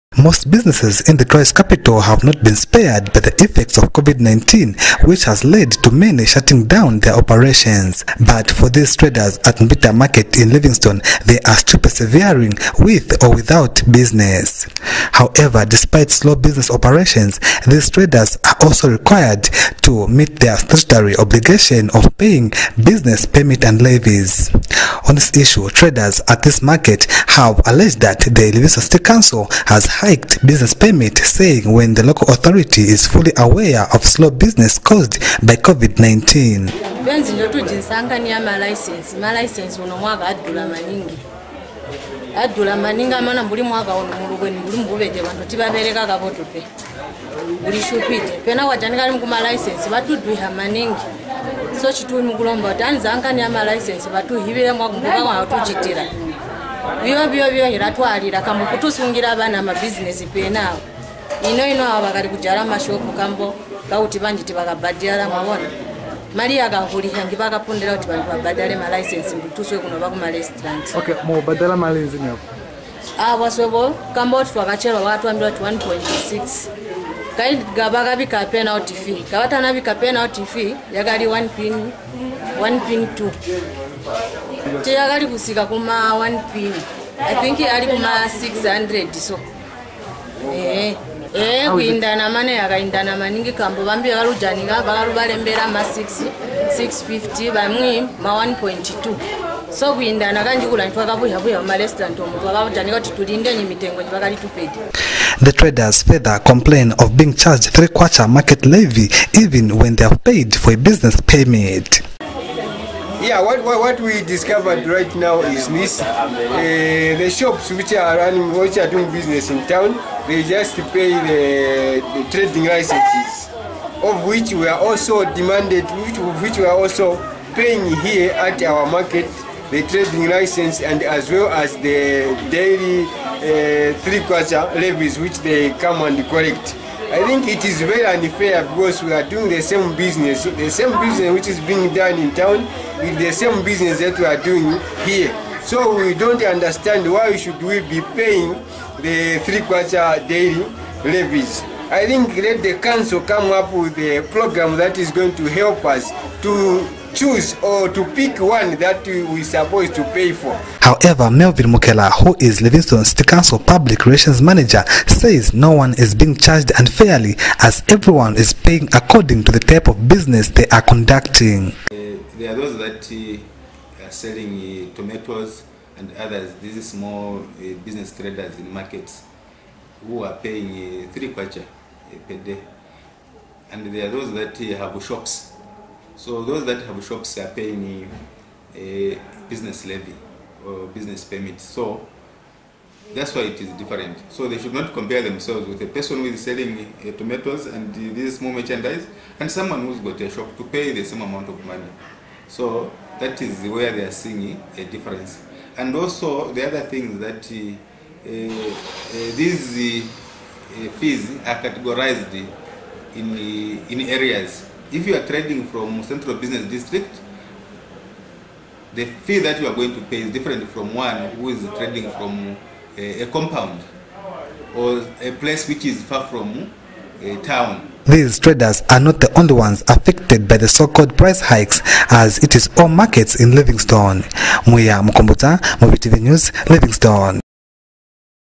Marketeers Unhappy With LCC’s Exorbitant Levies
Marketeers at Livingstone’s Mbita market have differed with the city council over what they term as exorbitant business permit and levies.

Marketeers have complained that the local authority has over hiked business permit fees.

But Livingstone city council public relations manager, Melvin Mukela says the local authority charged business permits according to the type of business one is operating hence the variation in fees.